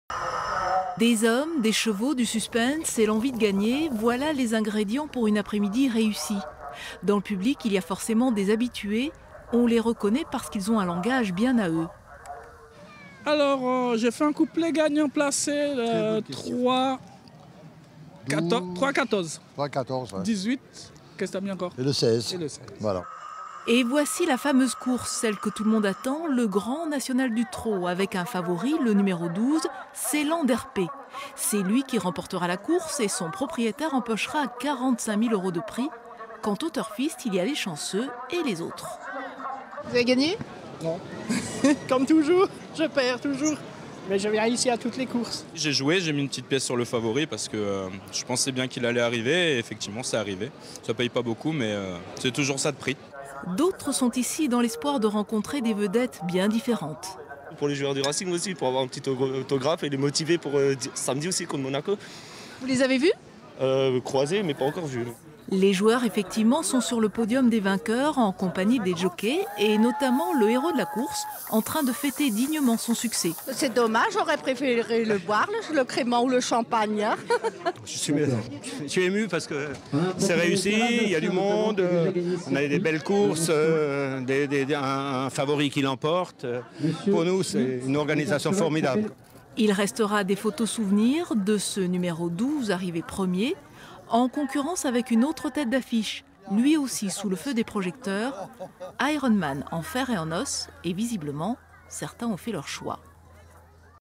Le Grand National du trot à l'hippodrome de Hoerdt
Événement à l'hippodrome de Hoerdt aujourd'hui : le Grand National du trot... Une course emblématique dans le milieu des turfistes et un Quinté très attendu. Il y avait donc beaucoup de monde : 3 000 personnes attendues. Des parieurs, mais aussi des gens veus en famille car de nombreuses animations étaient préves.

Sylvie Malal, Thierry Sitter